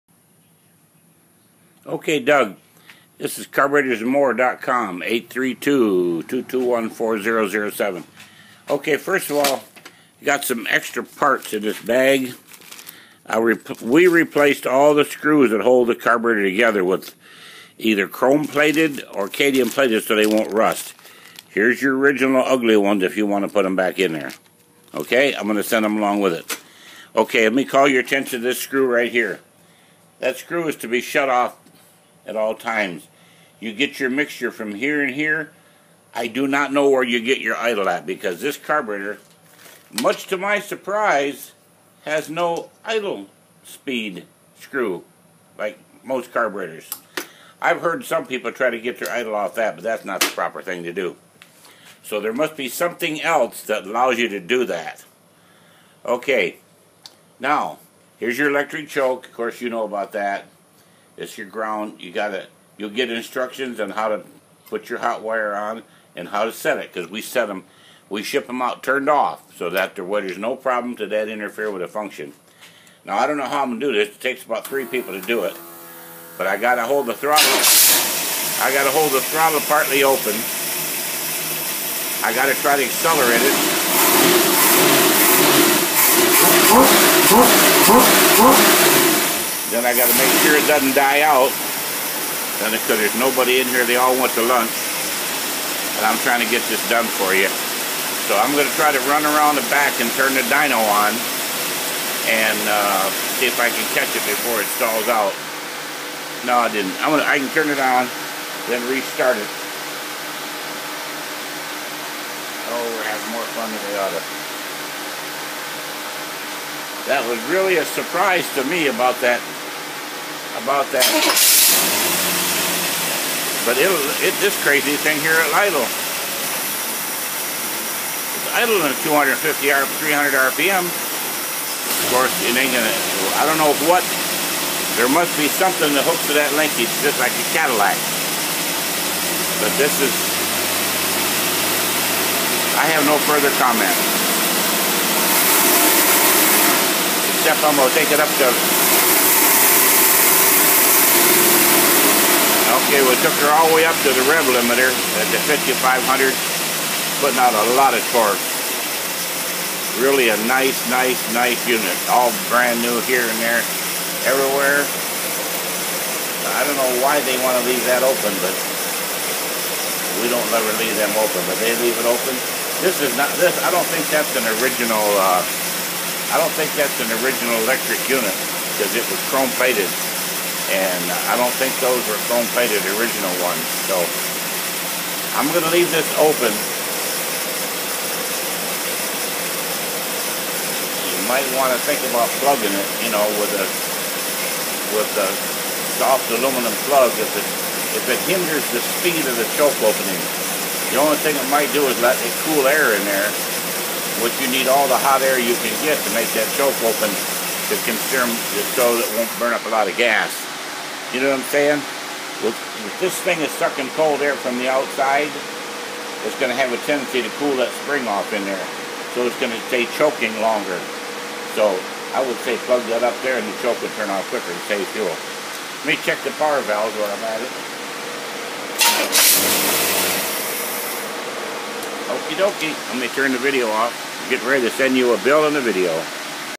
Doug Afb Carter
This is a Afb Carter that is being tested on a running engine.